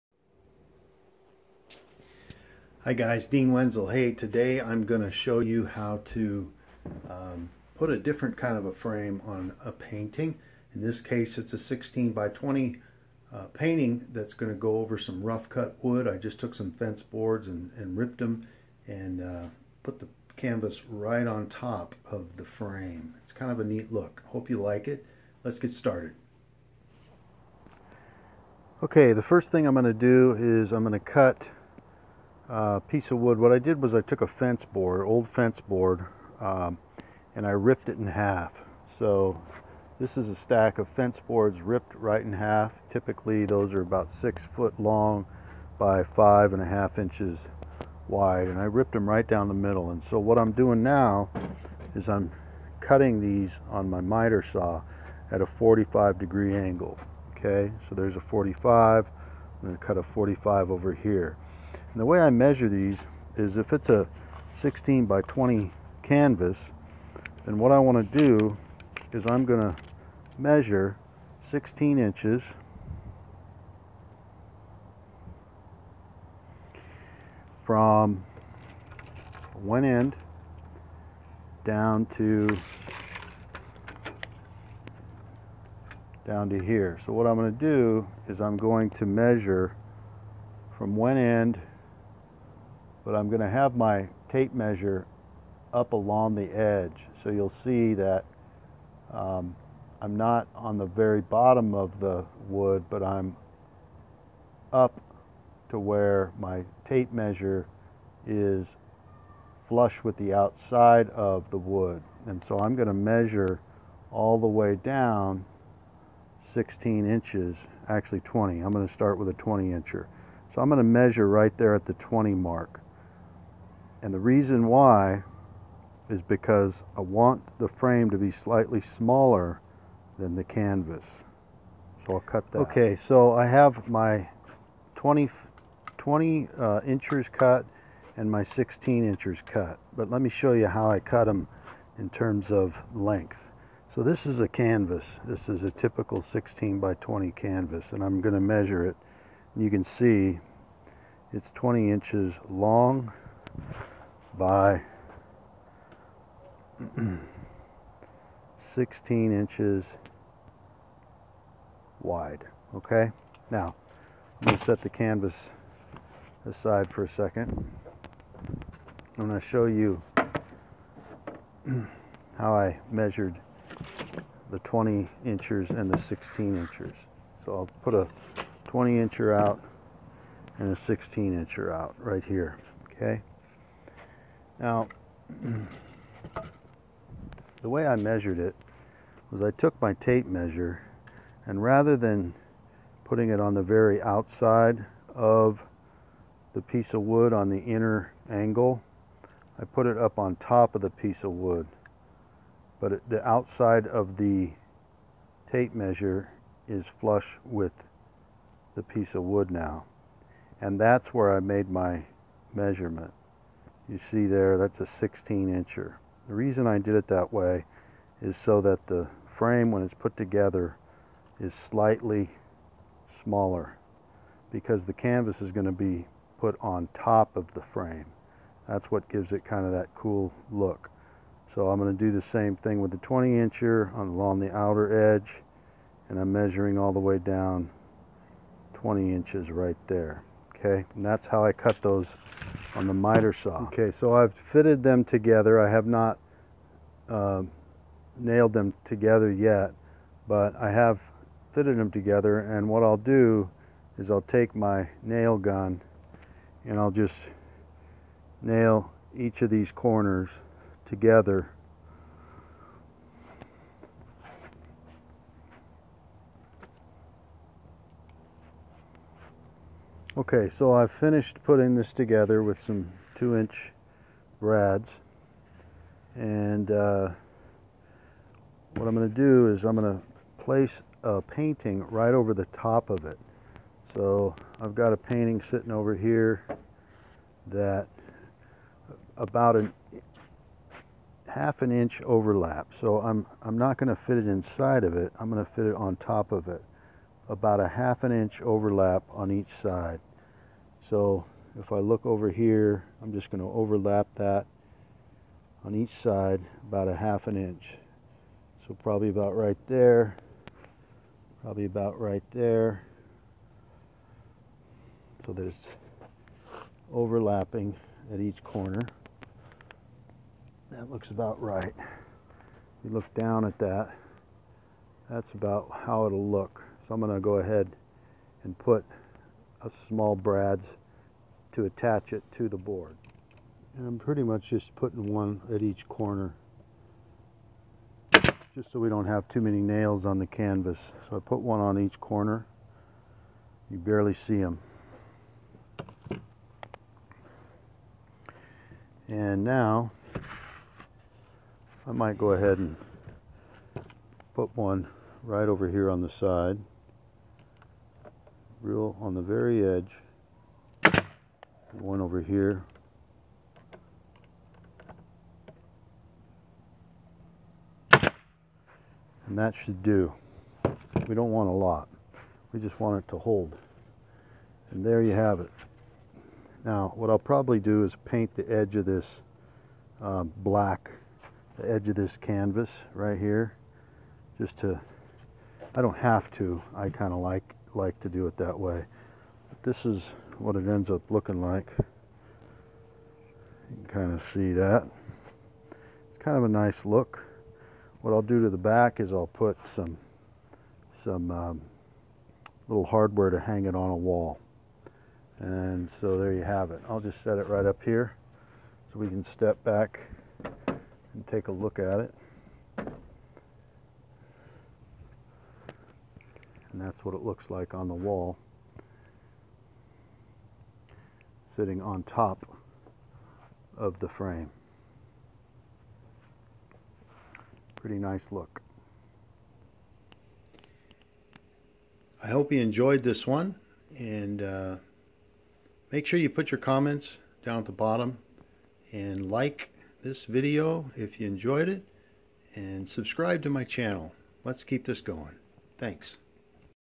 Turn your old fence into frames for your art!
My fence is getting old - why not use the old fence boards to add a cool touch to your paintings?  Here's how I do it.  Enjoy!